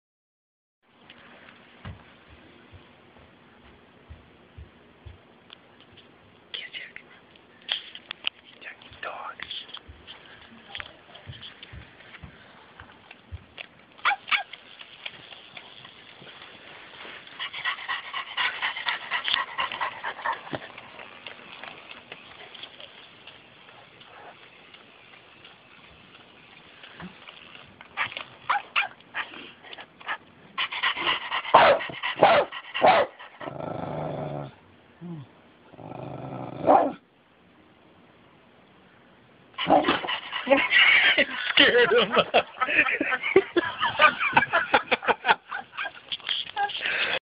Dog bumps head running from toy puppy.wmv
My dog Jack was being a tough little dog with my daughters toy puppy...until it made a sound and scared him! hahahaha!!